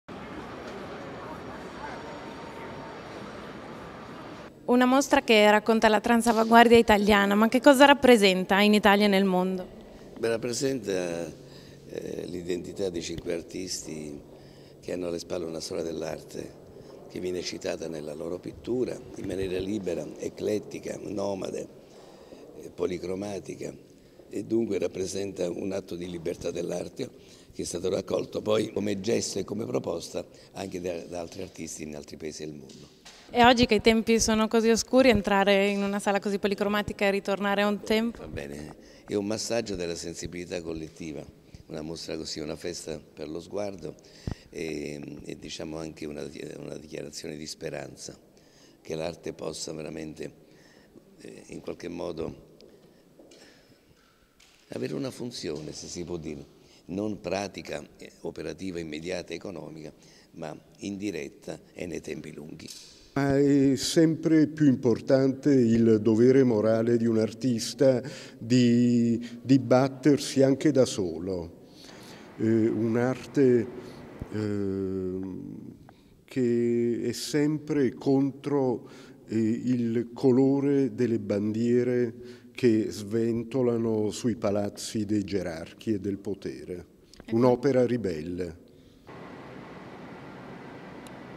A Palazzo Reale l'arte ribelle della Transavanguardia italiana
Milano. A Palazzo Reale, in occasione dei 150 anni dell'Unità d'Italia inaugura una grande collettiva, curata da Achille Bonito Oliva, che raccoglie insieme i protagonisti del movimento della Tra chia clemente cucchinsavanguardia: Sandro Chia, Francesco Clemente, Enzo Cucchi, Nicola De Maria e Mimmo Paladino. Tele colorate, spesso enormi, che illuminano e ispirano questi tempi bui. Servizio di Claudia Bellante